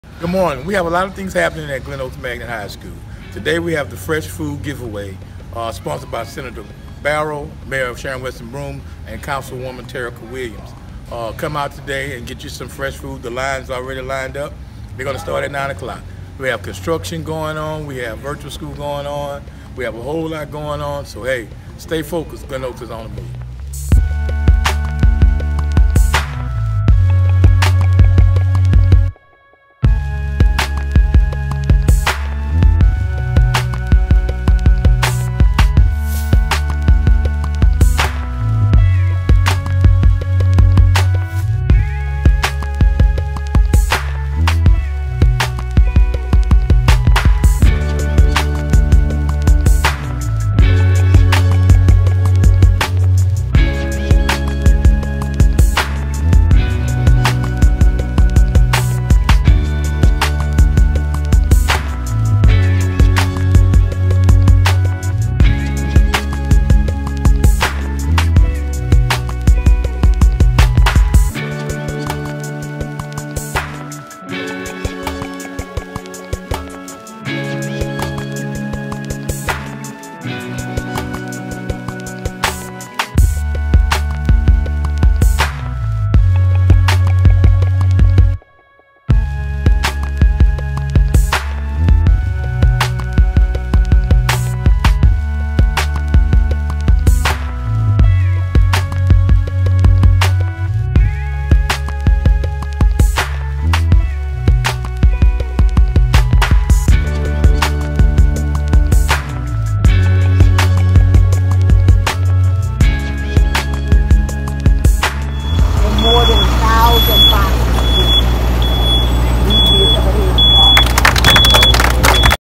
Fresh Food Give Away at Glen Oaks Magnet High School
Today our Glen Oaks Panthers joined @healthyBRcity, Mayor Broome, Senator Regina Barrow, Constable Terrica Williams, and served over 1,000 families Fresh Food!!